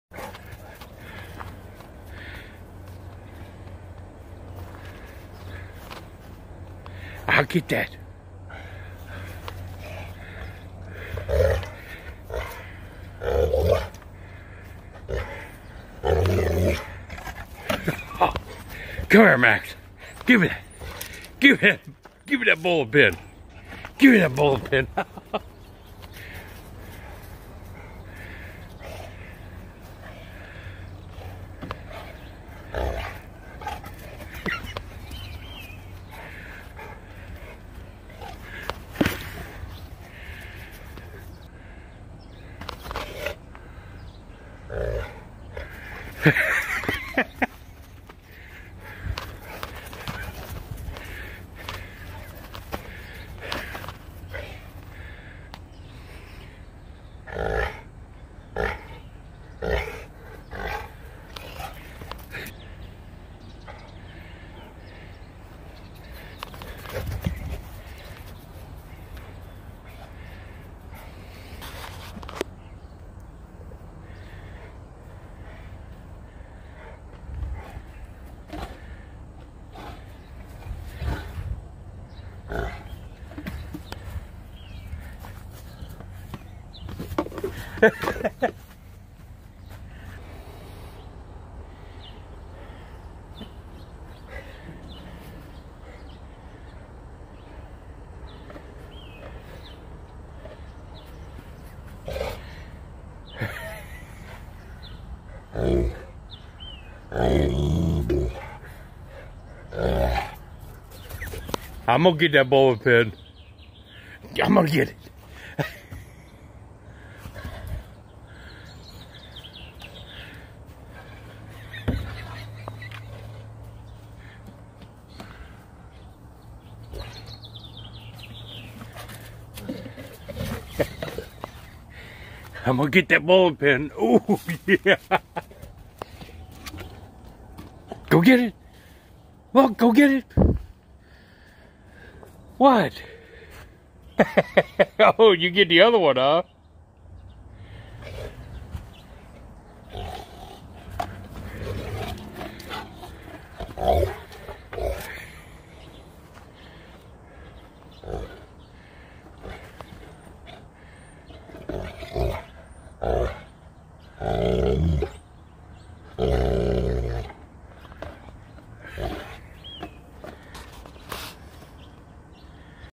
Saint Bernard Puppy Playing in 25 Degrees
I was working on the security cameras outside, and Max wanted to play lol.  It was 25 degrees and our Saint Bernard puppy loved it.